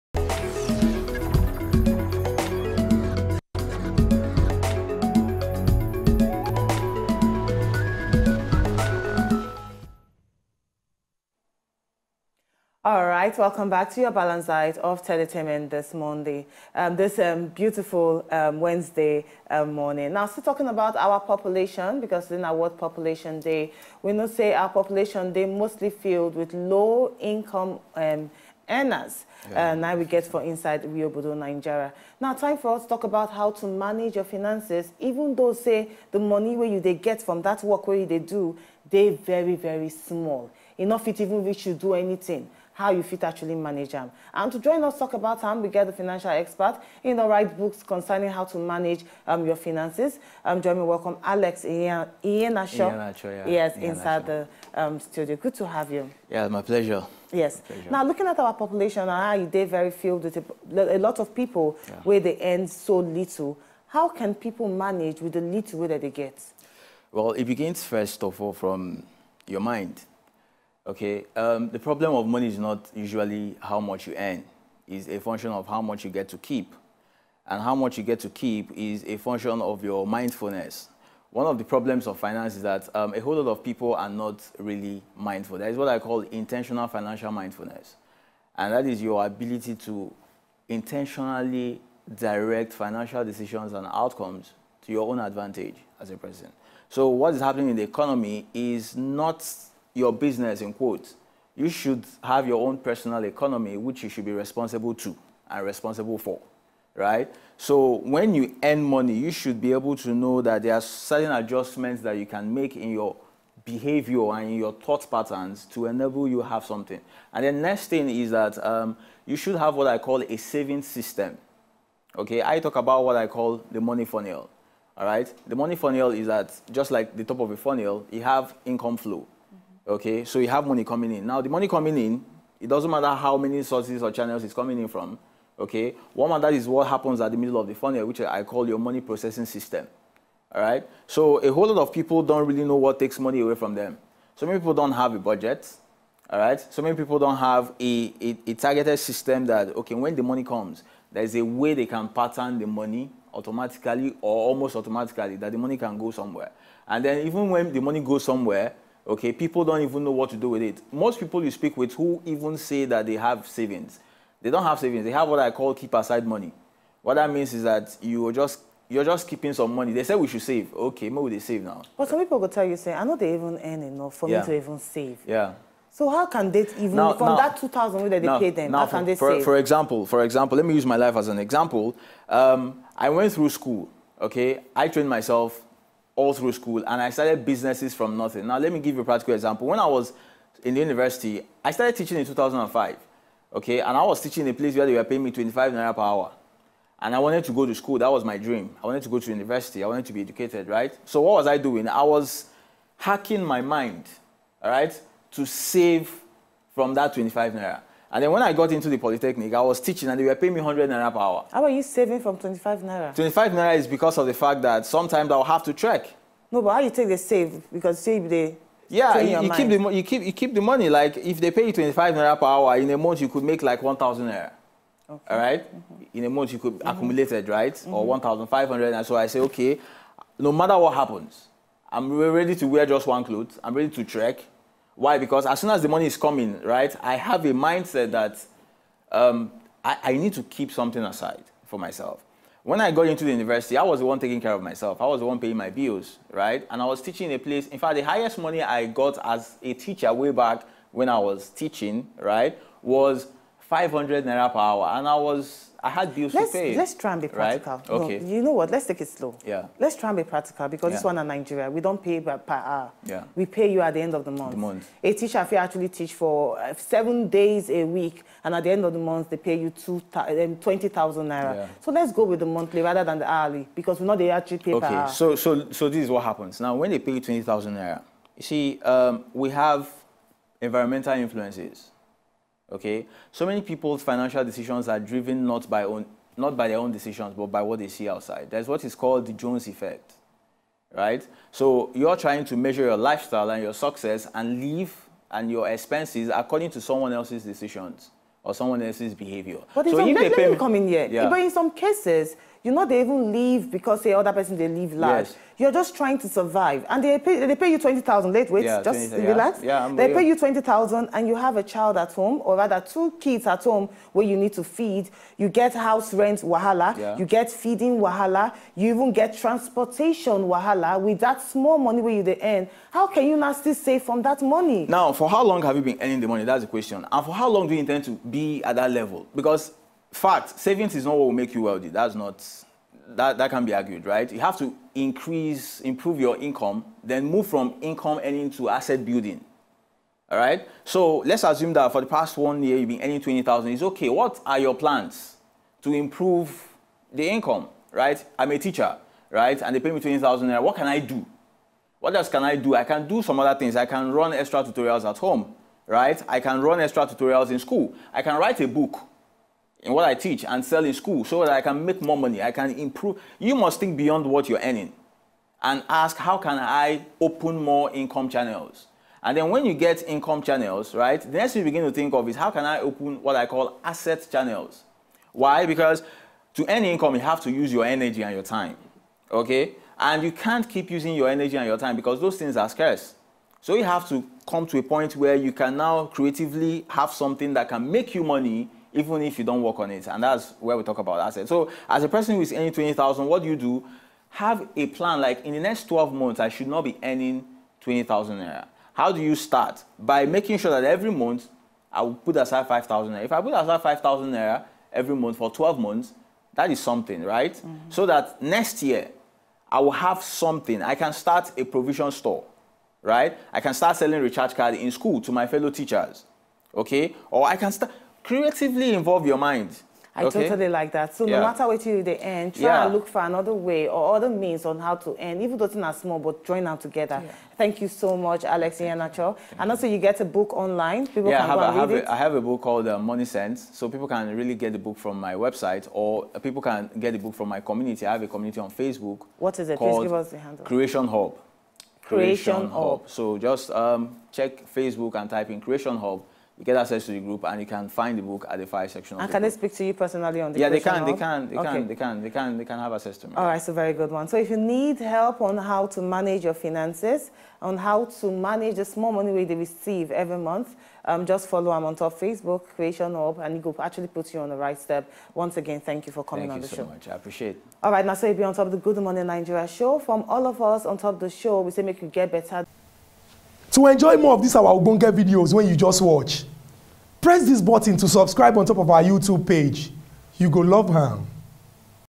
Tips on How to Manage your Finances - GMNS
Mr. Alex Ihenacho  ( personal finace coach) dishes out tips on how to manage our finances .